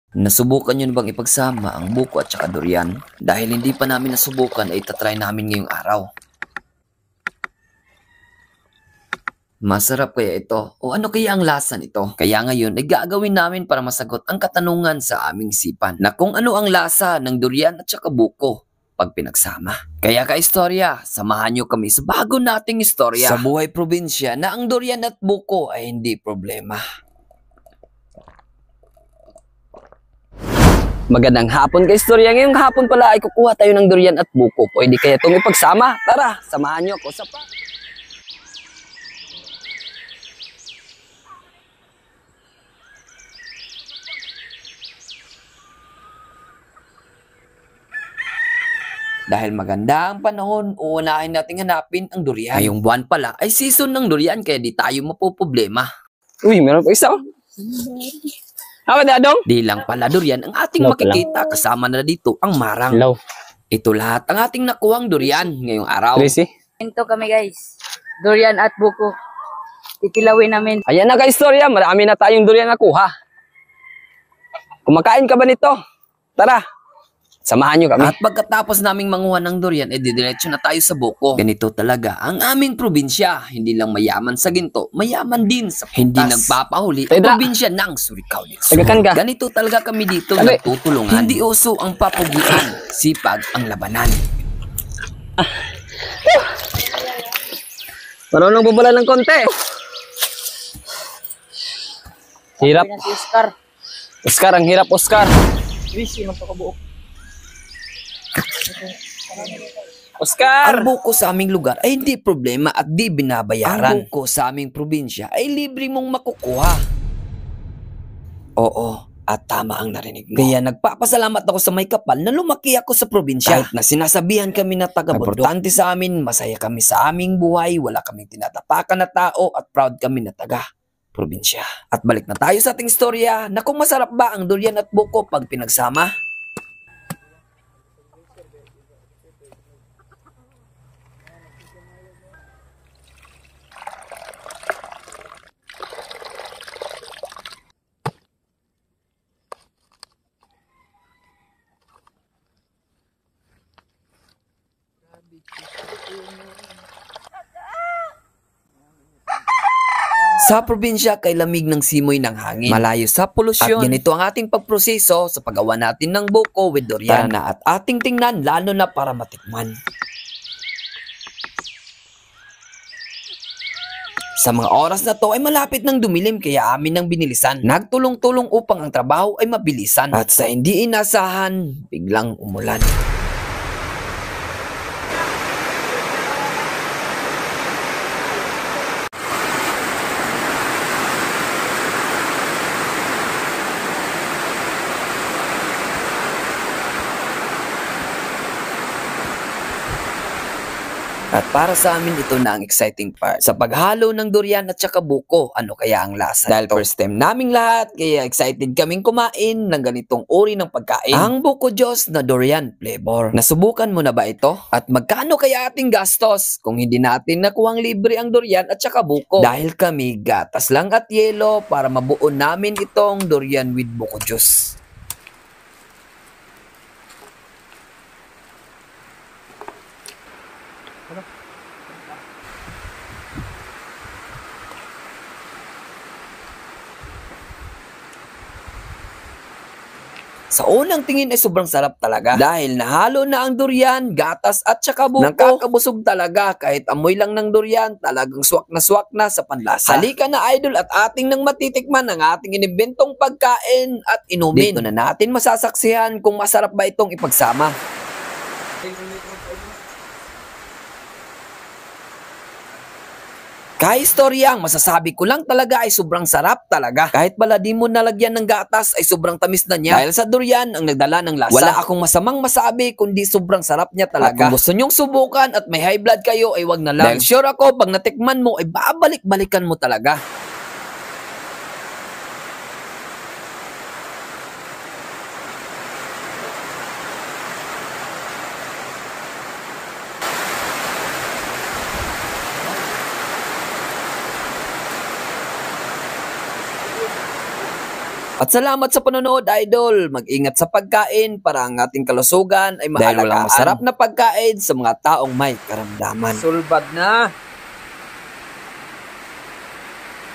Subrang sarap pala ng Buko Juice and DURIAN fruits pag pinagsama!
60 pesos na gastos sa mga Prutas na libre, kaya pinaghalo namin ang DURIAN at BUKO, ano kaya ang lasa nito?